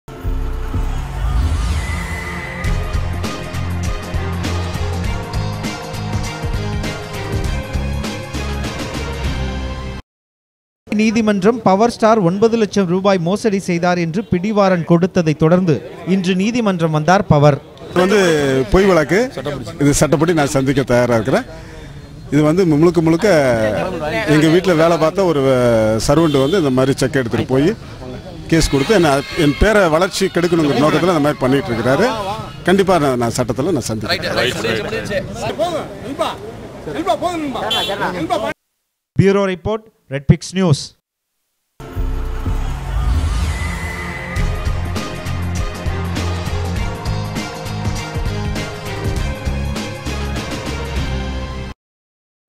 Power Star in Court [RED PIX]
Power Star in Court. Tamil Film actor Power Star appeared in Madurai court after he ws given an arrest warrant for a 9 Lakh cheque fraud case.